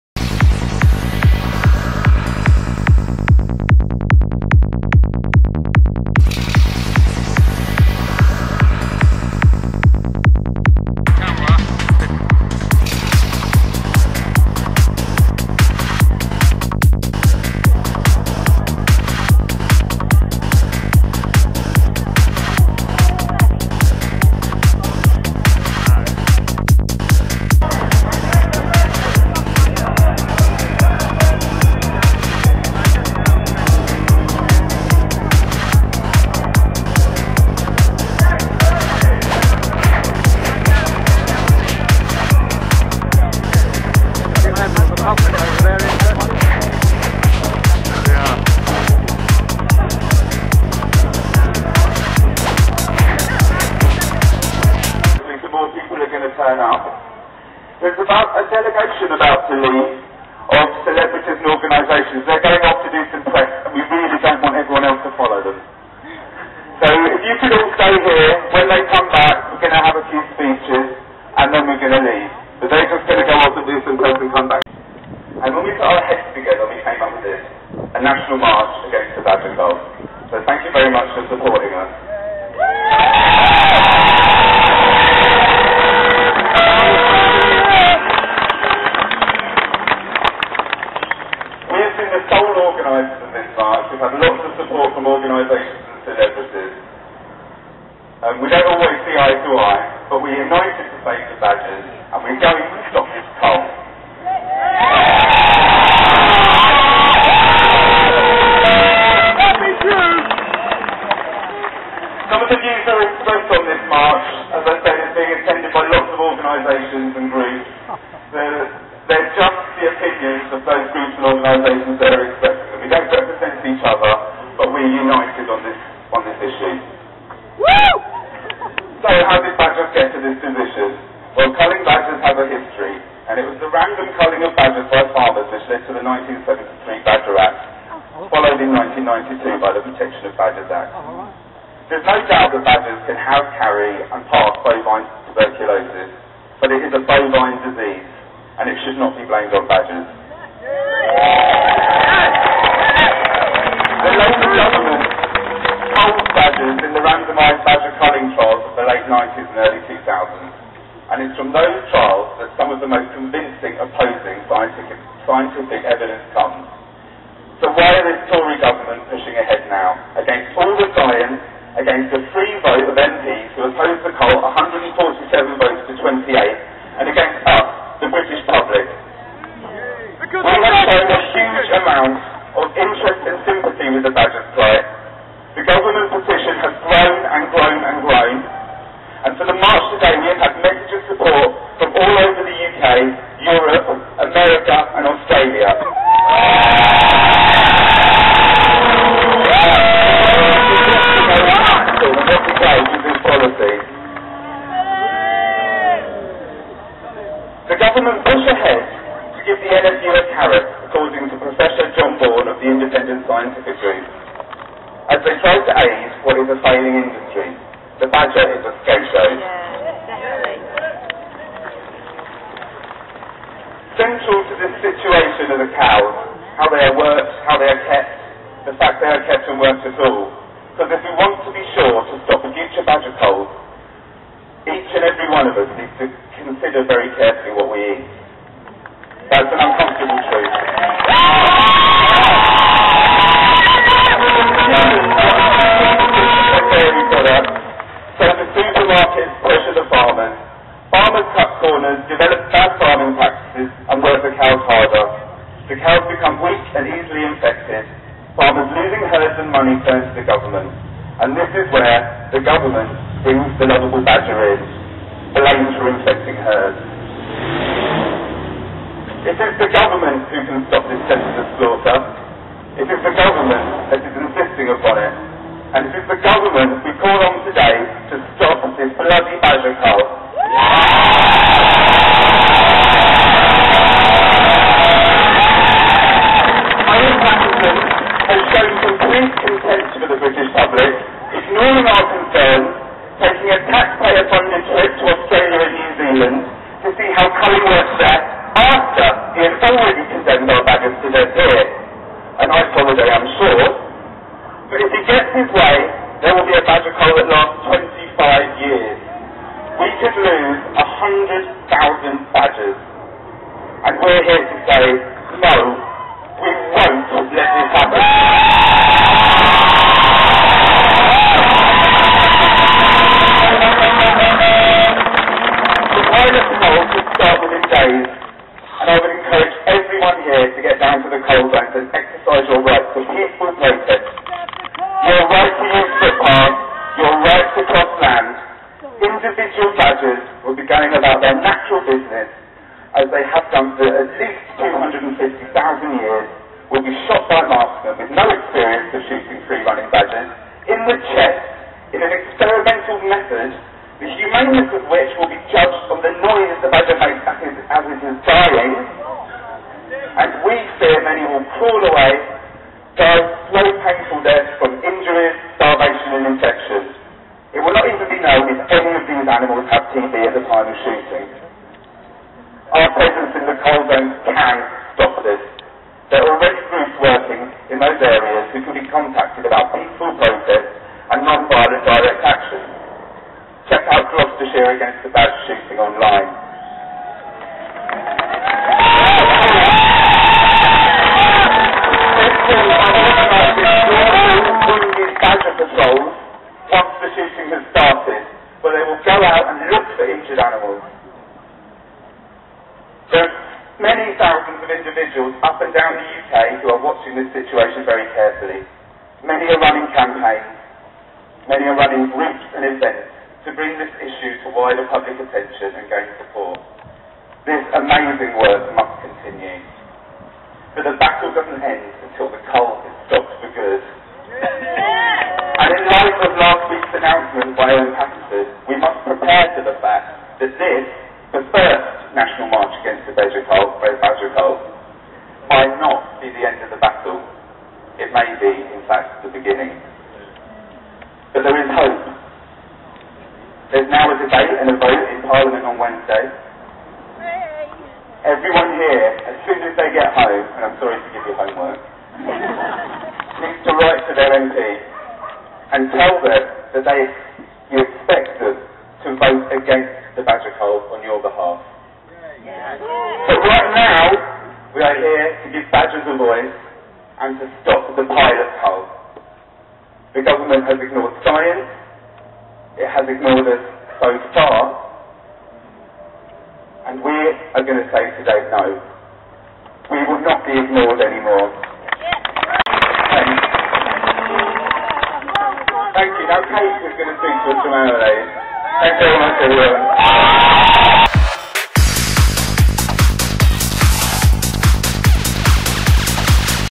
London Stop the Badger Cull Speech2
London Stop the Badger Cull Speeches June 1st 2013 March, Speeches by
Brian May of Queen, and Kate Fowler of Animal Aid, 5 separate
speeches, different wmv files.